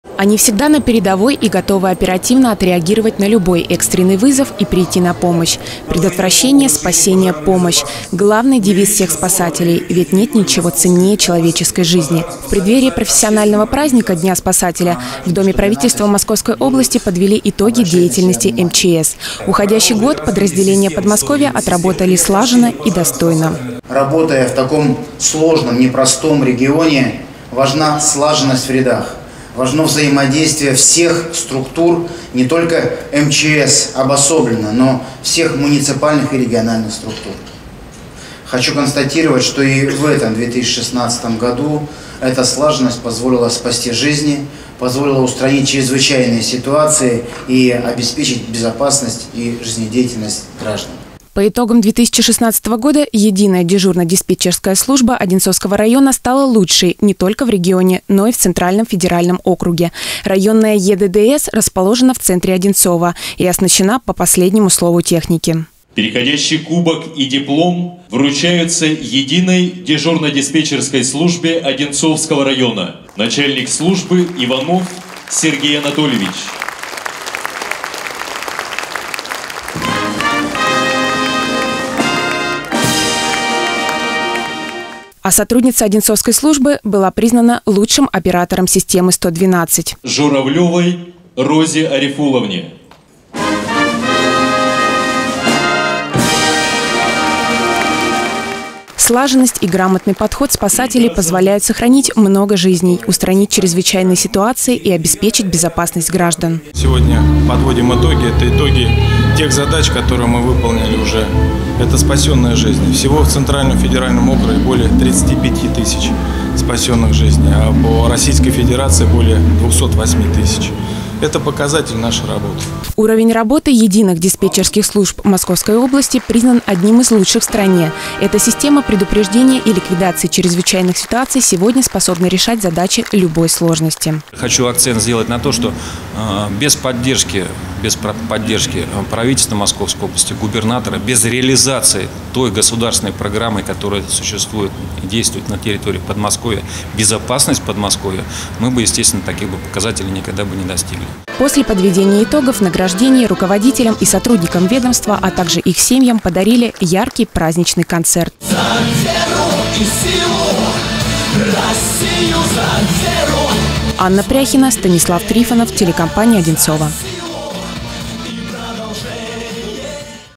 Одинцовская ЕДДС признана самой эффективной
В Доме правительства Московской области подвели итоги деятельности подмосковной системы предупреждения и ликвидации чрезвычайных ситуаций. Особо отличившихся руководителей и сотрудников ведомства наградили орденами, медалями и грамотами. Среди победителей и Одинцовская Единая дежурная диспетчерская служба.